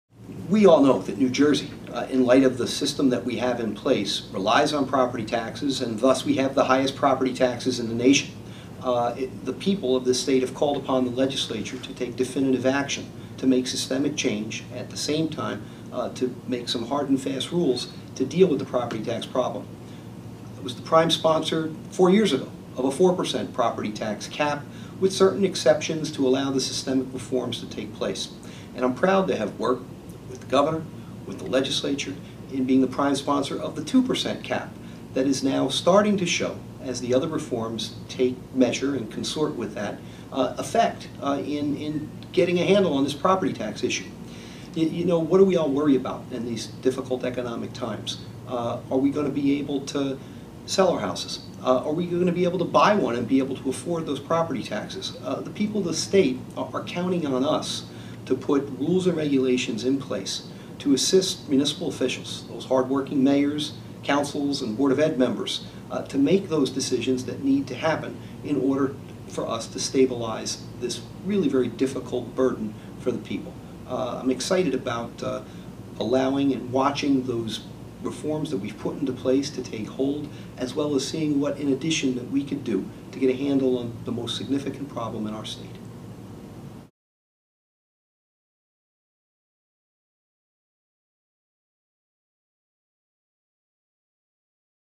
One on One with John McKeon: Taxes
Sitting down one on one with Assemblyman John McKeon of New Jersey's 27th Legislative District to talk about stabilizing property taxes in NJ.